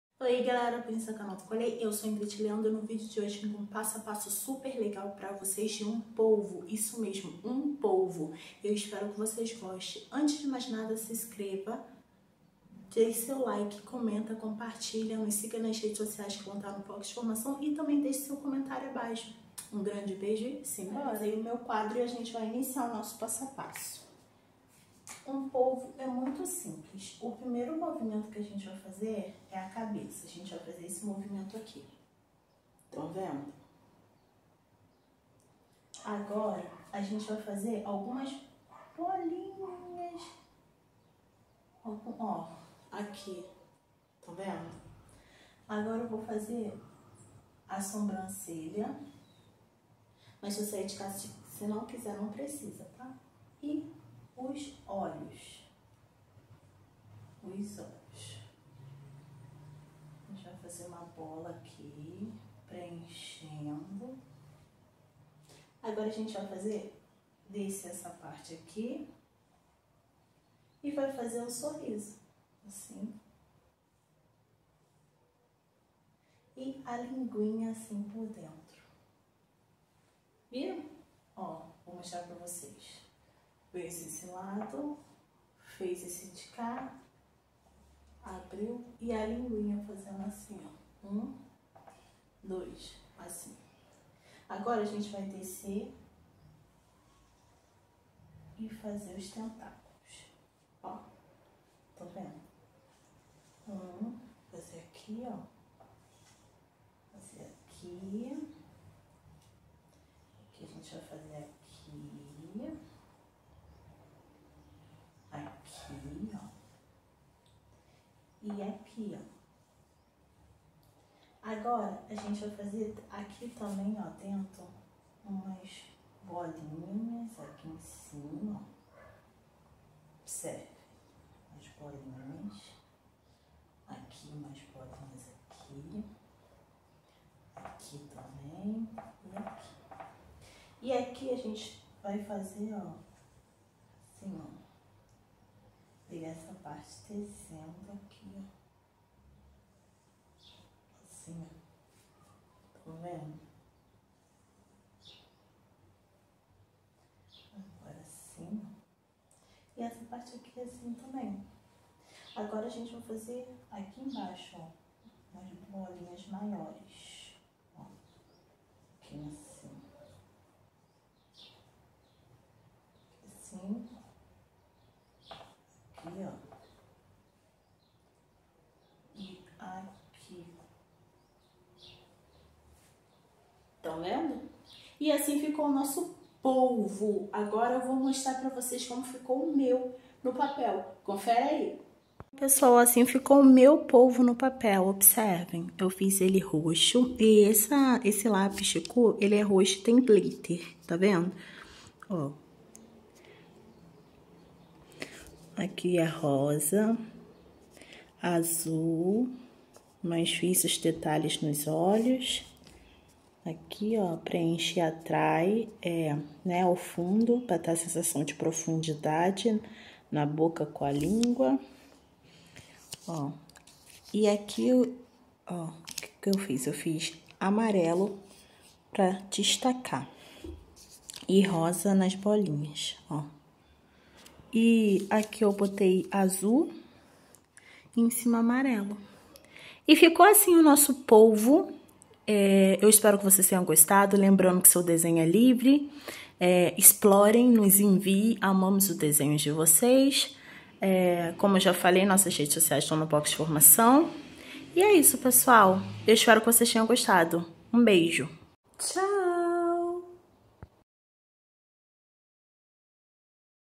Desenho passo a passo O polvo por Ingrity Leandro
Somos o Coletivo de Estudos e Pesquisas sobre Infâncias e Educação Infantil (COLEI), e a mediação de hoje é feita pela Bolsista Ingrity Leandro.

* Inspiração do Pinterest.